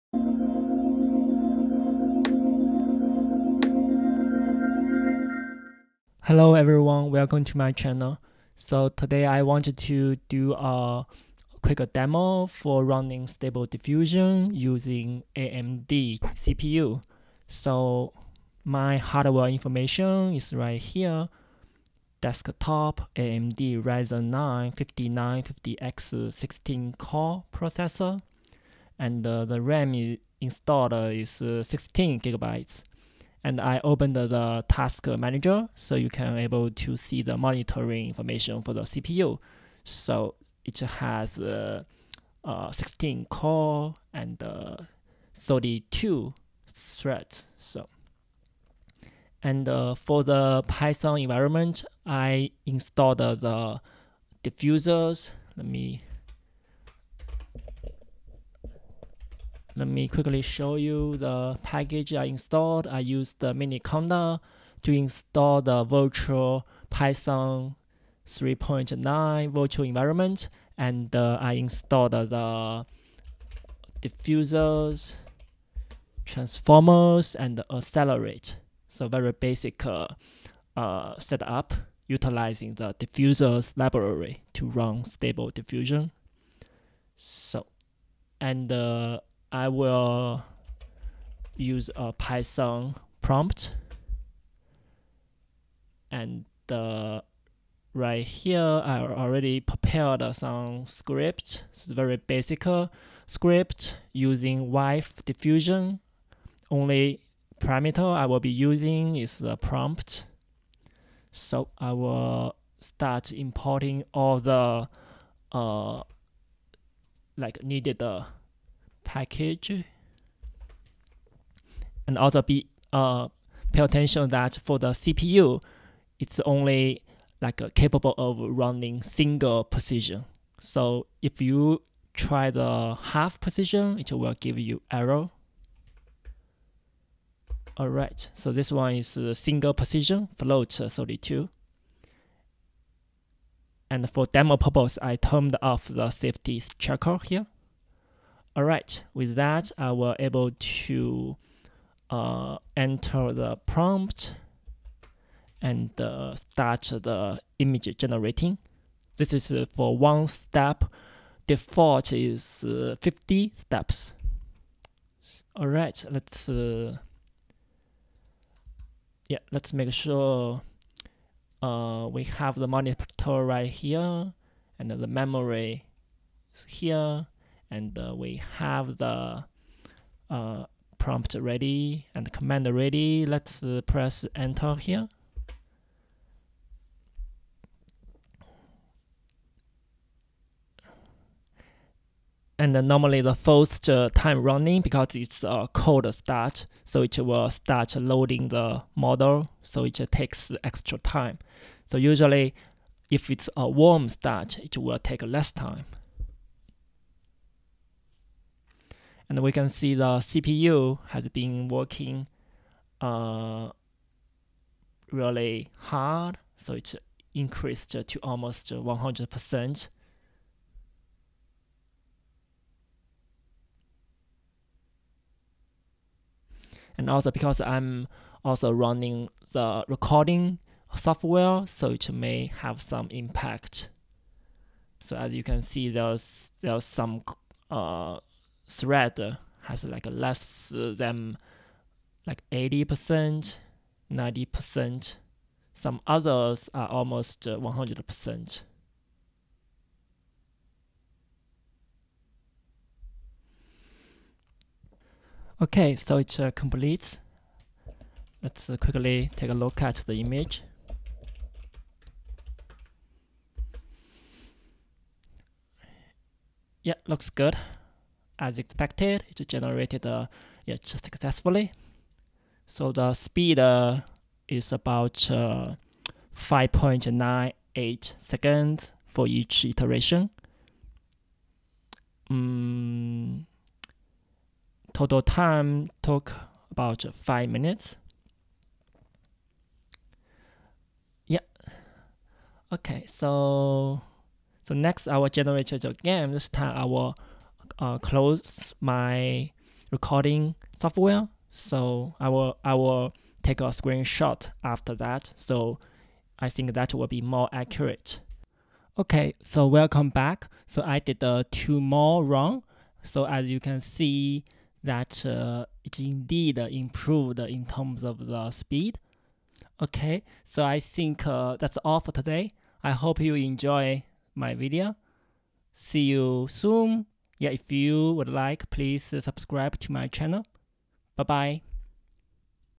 16-core CPU - AMD 5950x running stable diffusion to turn text into image
In this video, a quick demo of running stable diffusion using AMD CPU 5950x is shown. The model used is waifu diffusion v1.4
It's not as fast as GPU, but few minutes (less than four minutes per image for a 51 steps default setting image) is still acceptable IMO compared with manual paintings.
1:00 python virtual environment
1:40 start executing stable diffusion code
2:45 first test up and running
4:13 first run result
5:05 additional two runs